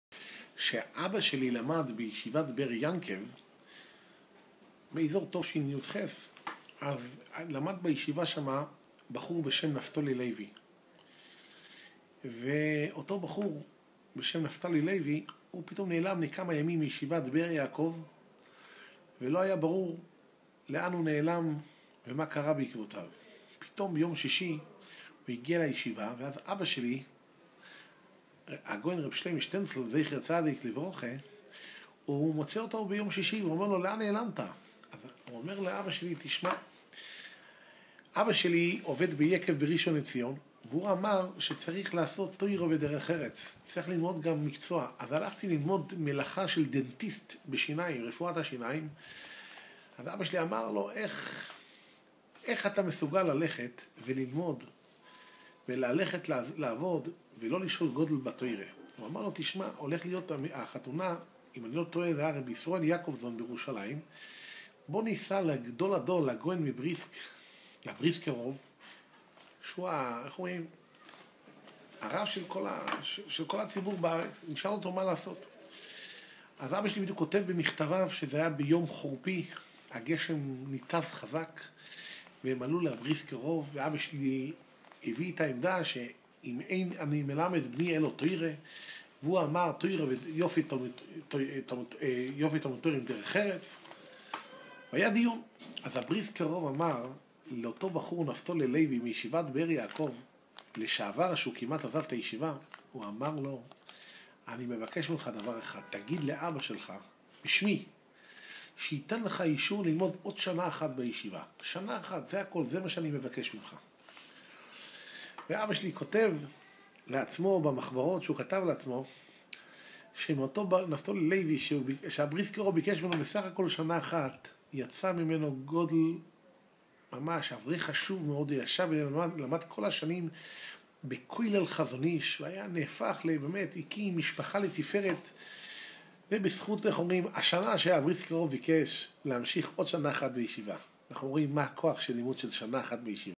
איך שכנע הרב מבריסק את תלמיד הישיבה הרב נפתלי לוי לחזור ללמוד ב ישיבת באר יעקב?
תלמיד ישיבת באר יעקב הלך כמצוות אביו ללמוד את עבודת דנטיסט של לימודי שיניים, ועזב את ישיבת באר יעקב, חברו לספסל הלימודים שכנע אותו ללכת לשאול את הגאון מבריסק,  האם לפעול כמצוות אביו של לוי יפה תלמוד תורה עם דרך ארץ, או ללכת בשיטת "ואיני  מלמד את בני אלא תורה", אותו תלמיד שמע את עצת הבריסקר רוב, ללמוד עוד שנה בישיבה, והפך לתלמיד חכם עצום,  ב כולל חזון איש בבבני ברק, ונפטר לפני מספר שנים.